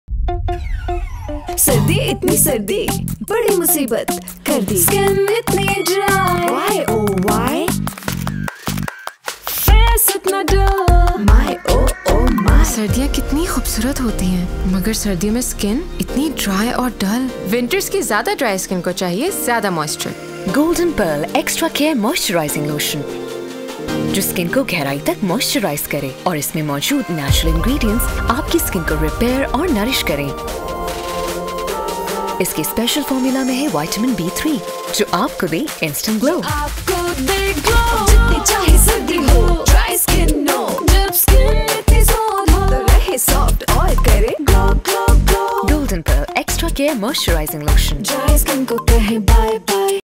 Golden Pearl Body Lotion 50s Film Featuring Sana Javed
Directed by: Shahid Iqbal